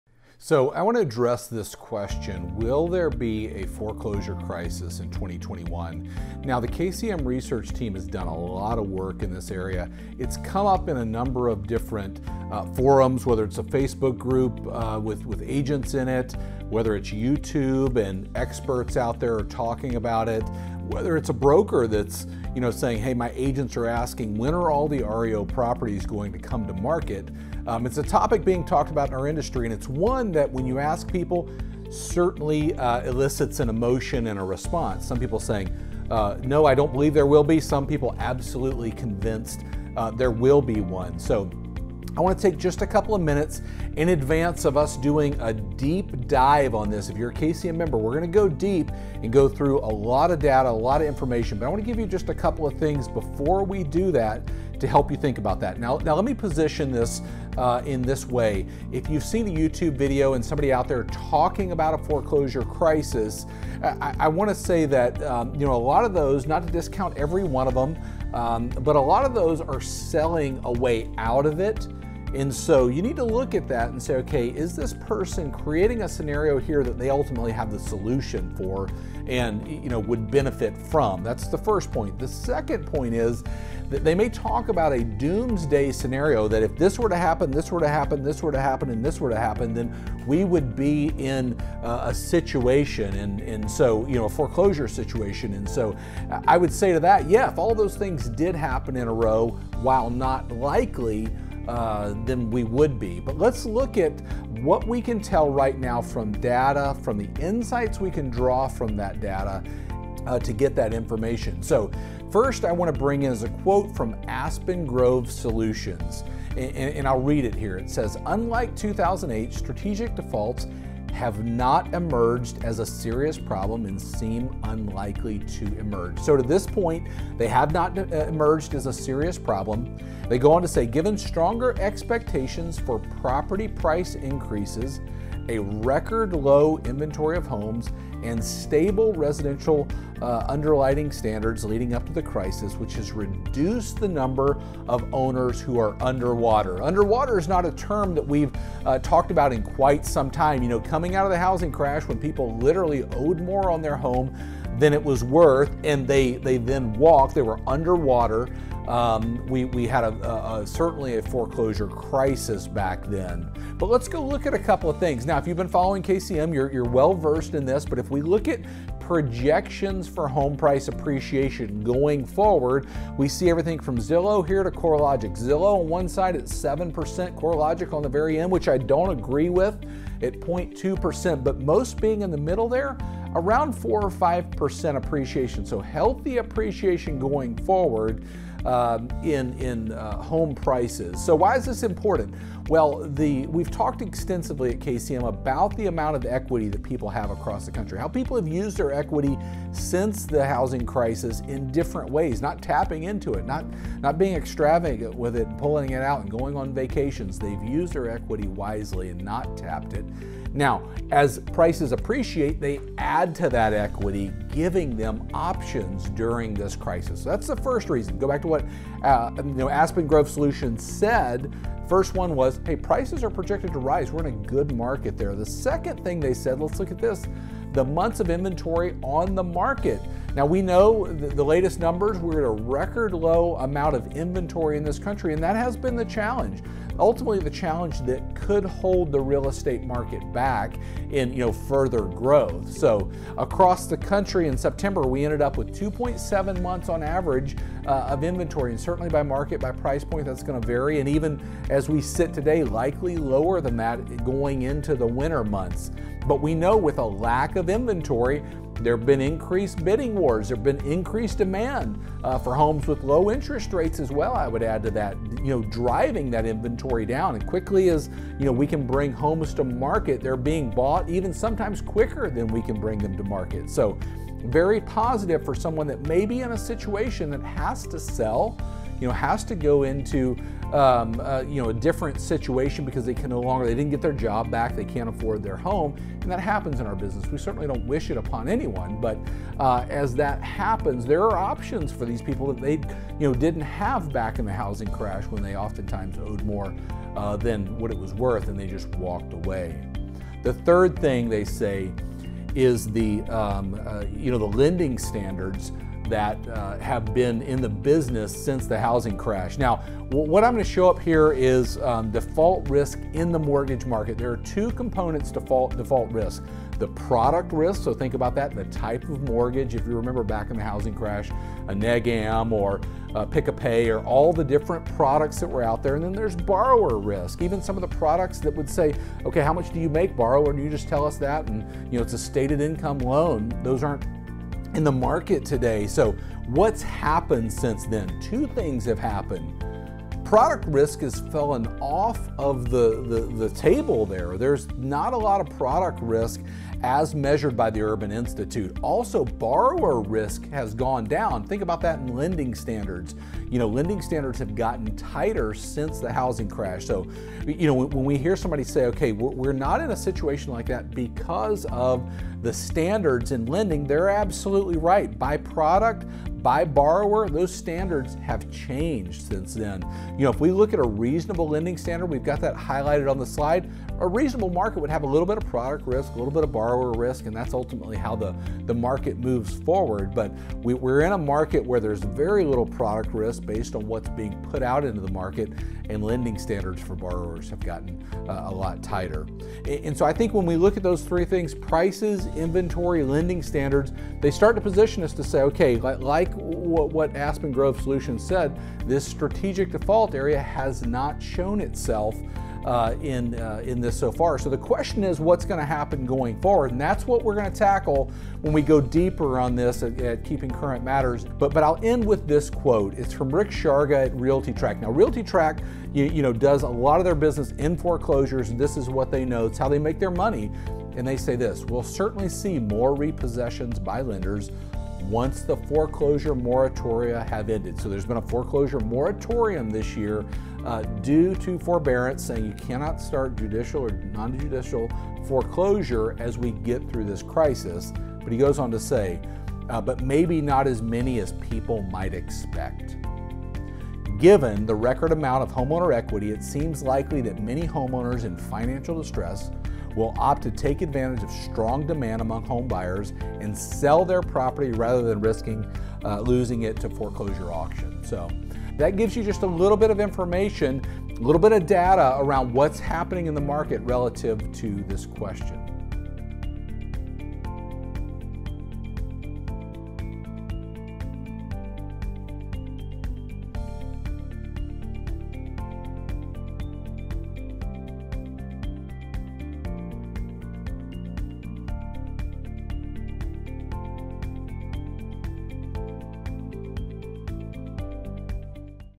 Why We are Not Headed Toward a Foreclosure Crisis | Keeping Current Matters
There seems to be some concern that the 2020 economic downturn will lead to another foreclosure crisis like the one we experienced after the housing crash a little over a decade ago. In this video, David Childers breaks down some of the key data suggesting we are NOT headed toward a Foreclosure crisis.